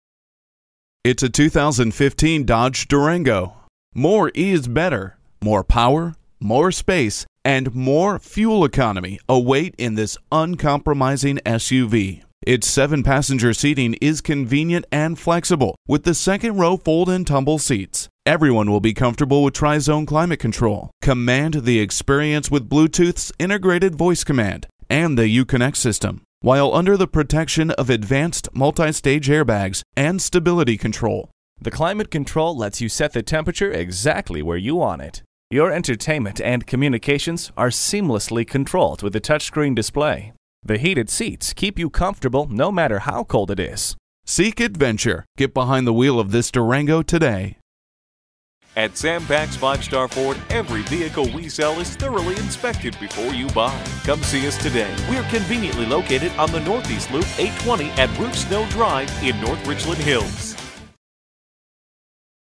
Used 2015 Dodge Durango North Richland Hills, TX
Year: 2015
Make: Dodge
Model: Durango
Trim: R/T
Engine: HEMI 5.7L V8 Multi Displacement VVT
Transmission: 8-Speed Automatic
Color: Gray
Interior: Black
Mileage: 18708
Stock #: GL225096A
VIN: 1C4SDHCT4FC786507
** ONE OWNER ** NAVIGATION ** LEATHER ** HEATED SEATS ** REARVIEW CAMERA ** REMOTE START ** Don't pay too much for the SUV you want...Come on down and take a look at this fully-loaded 2015 Dodge Durango. This outstanding Dodge Durango is just waiting to bring the right owner lots of joy and happiness with years of trouble-free use.

Address:
6618 NE Loop 820